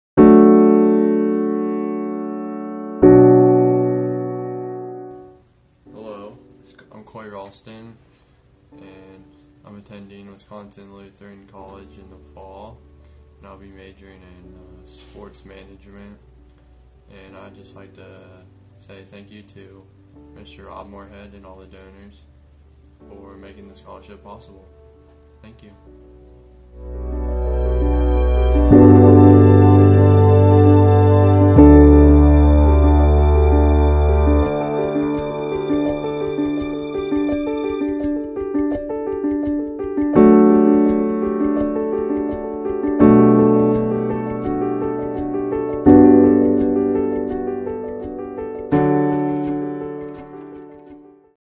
The Moorhead Student Athlete Scholarship "Thank You" Video
Cory Ralston, the 2020 Moorhead Student Athlete Scholarship Recipient, gives a special message to the donors.